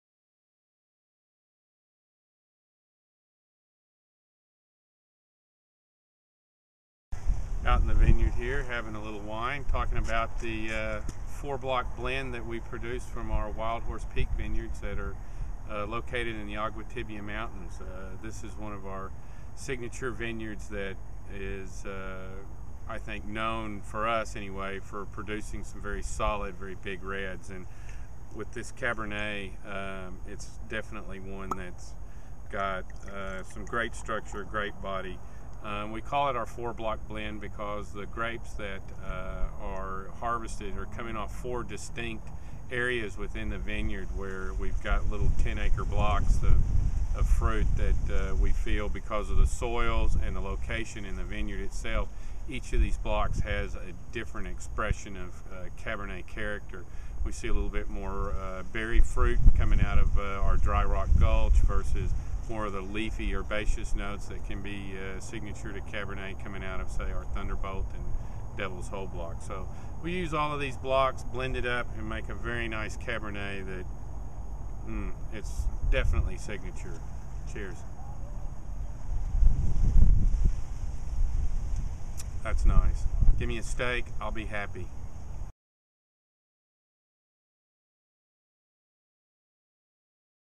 Bottle of the Week - Week 2: Wild Horse Cabernet Sauvignon
South Coast Winery Master Winemaker Jon McPherson talks about the origins of this big red California Cabernet